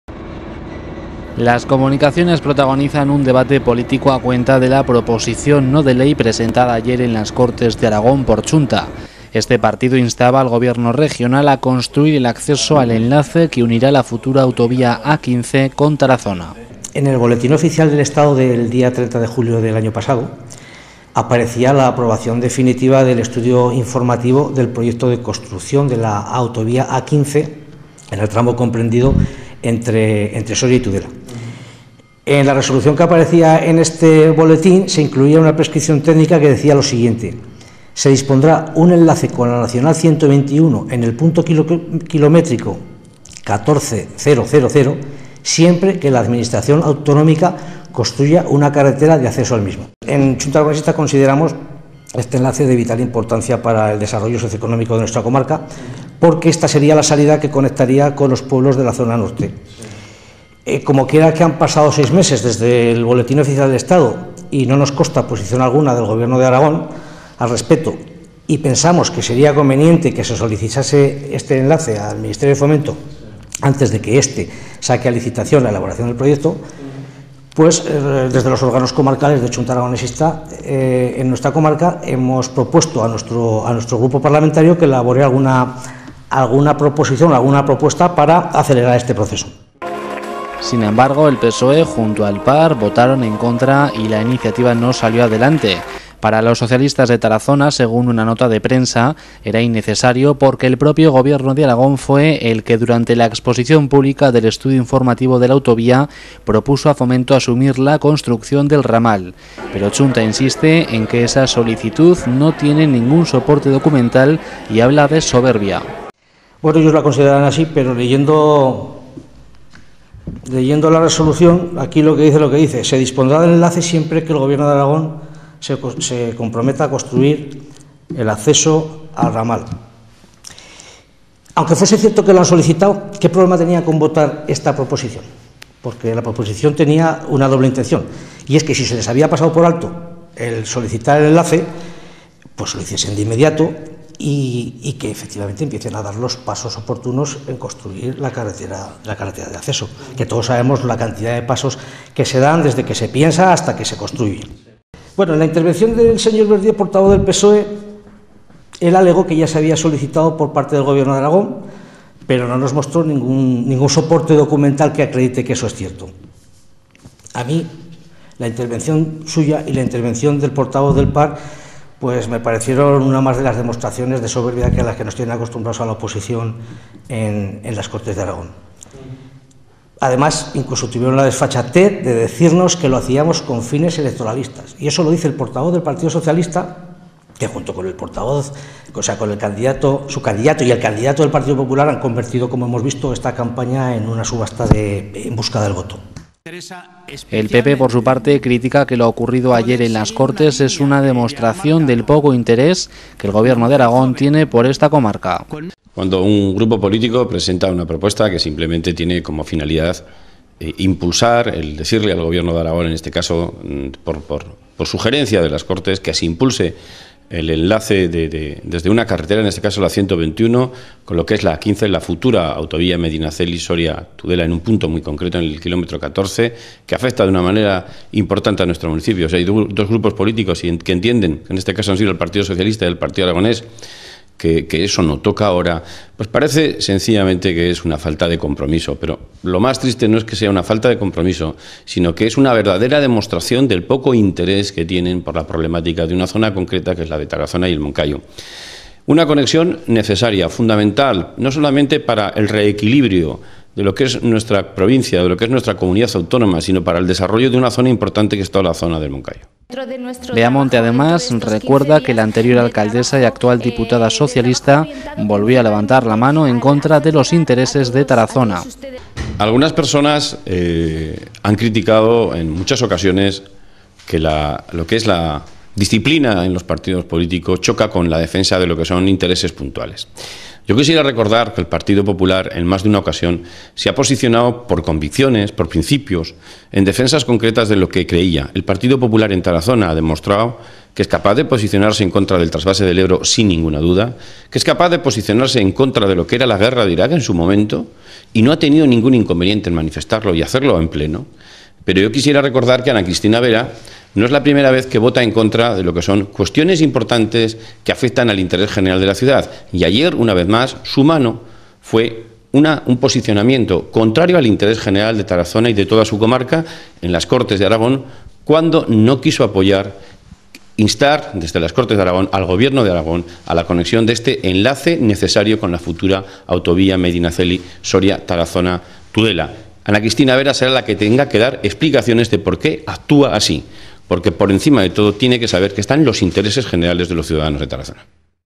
PSOE y PAR rechazan en las Cortes el acceso a la A-15
Votaron en contra de una proposición no de ley presentada por CHA para instar al Gobierno de Aragón a construir el acceso al enlace entre Tarazona y la futura A-15. Al PSOE le parecía "innecesaria". Para el PP, el presidente Marcelino Iglesias "margina" a Tarazona.